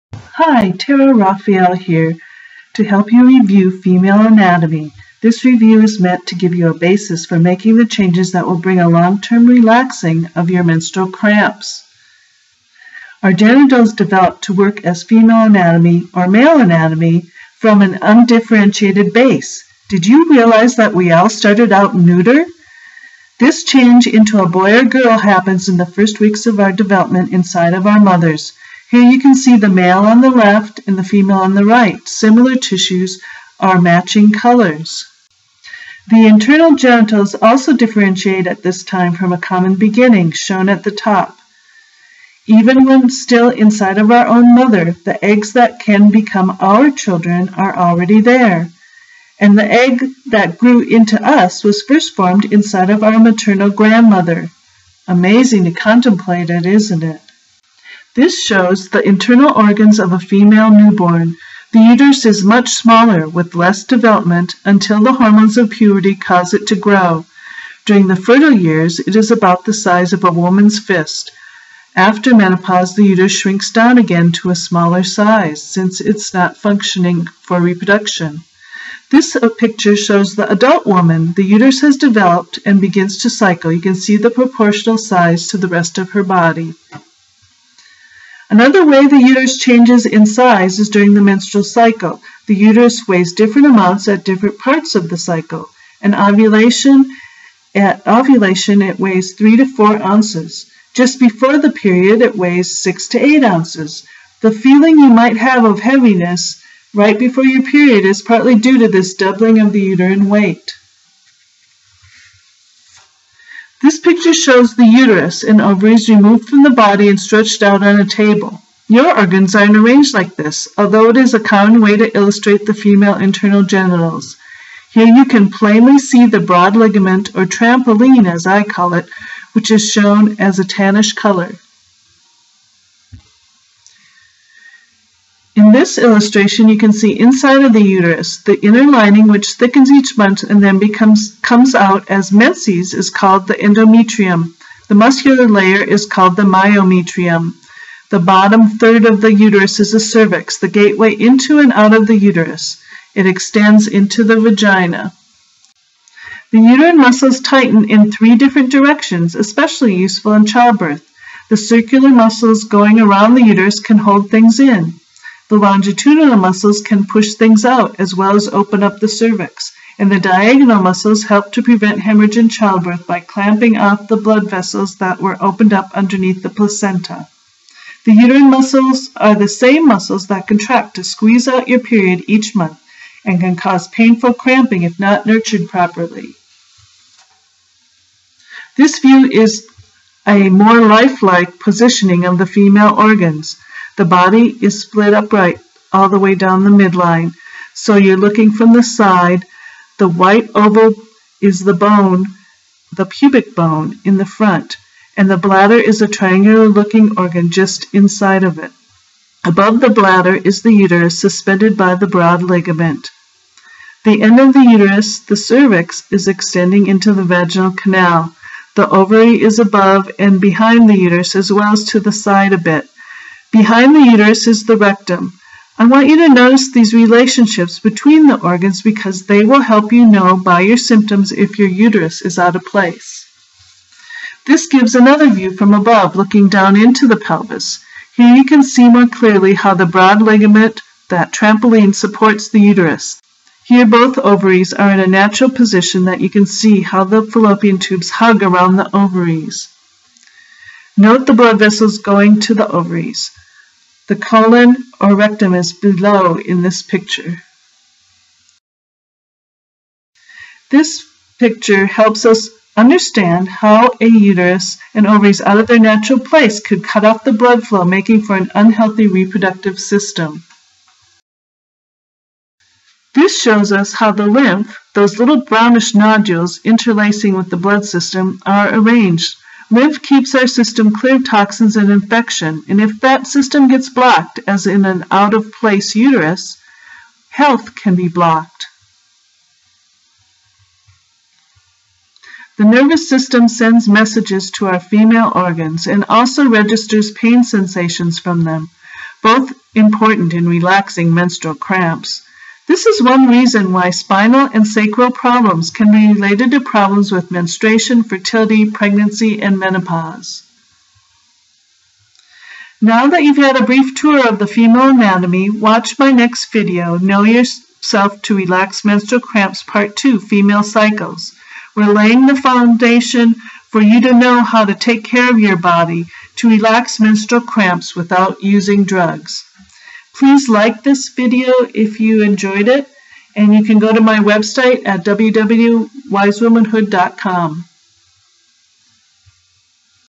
Female Anatomy
Review the basics of female anatomy to begin to understand how to relax menstrual cramps, be fertile naturally and surf menopausal changes.  For more classes by Terra Rafael, WiseWomanhood Holistic Womens Health